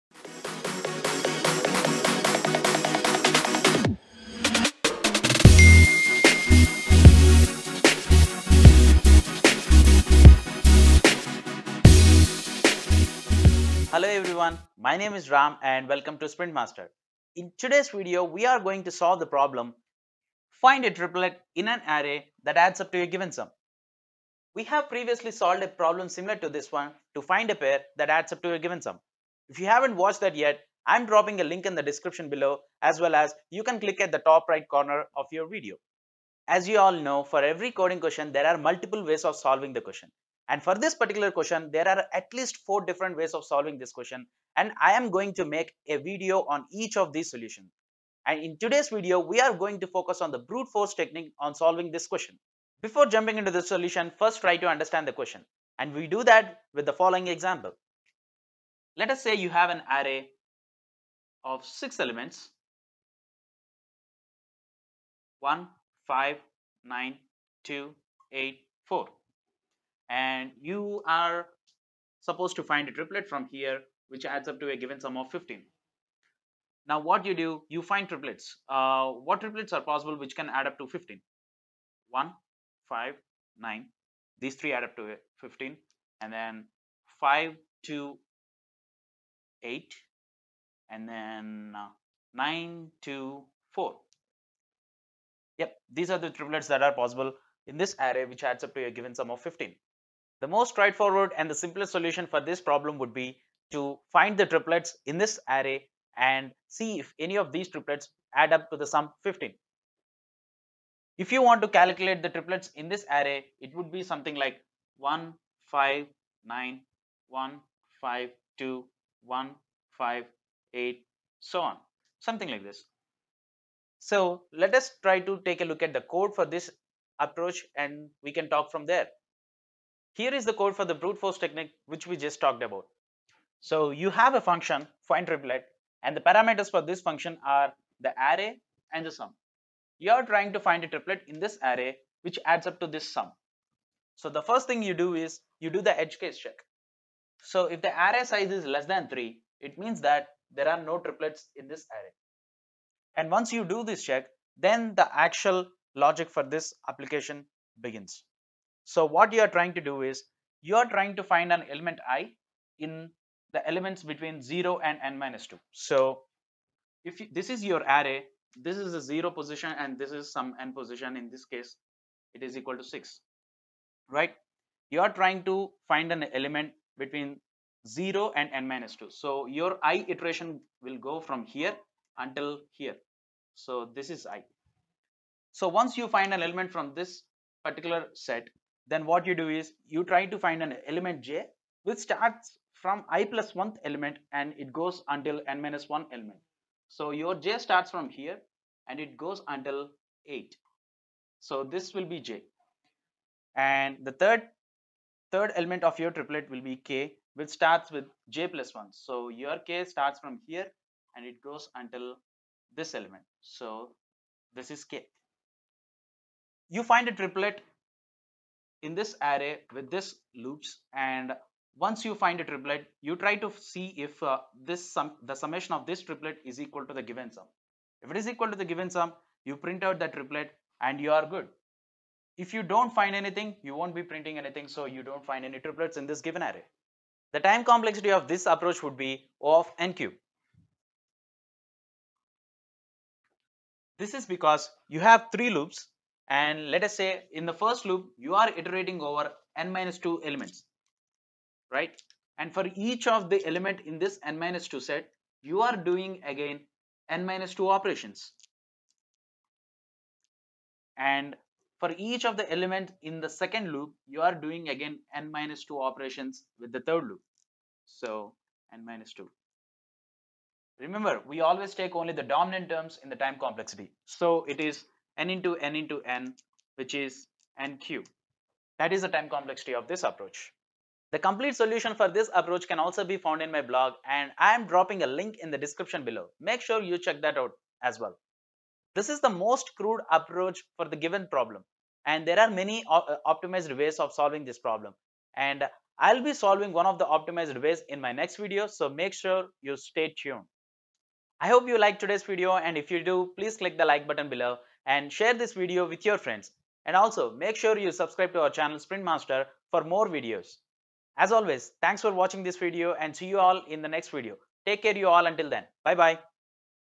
Find all triplets in an array with a given sum | Brute Force Technique
In this video, we are solving a coding question, "Find all the triplets in an array that add up to a given sum".

We have previously solved a question similar to this one in our previous videos, which is to "Find a pair that adds up to a given sum", and you can watch that video in the following link.

When you face interviews with Big Tech companies like Google, Amazon, Facebook, Microsoft, Apple, you may face this kind of questions.
For any coding question, there isn't a single approach or a single solution to solve the problem. There will be multiple ways, and which approach we are going to take purely depends on the complete picture of the question.
This particular question of finding triplets with given sum can be solved in at least four different ways. I am going to show you how you can solve in 4 different ways in this video, and the next upcoming videos.

In this video, we focus on the easiest and most obvious solution to this problem (Brute-force technique) which is to find all the triplets that the array can produce, and checking if any of those combinations add up to the given sum.
The time complexity of this approach is O(n^3) as we iterate over the array 3 times to get each element from the array to form a triplet.

You can find the complete solution in my blog as well, and here is the link for it.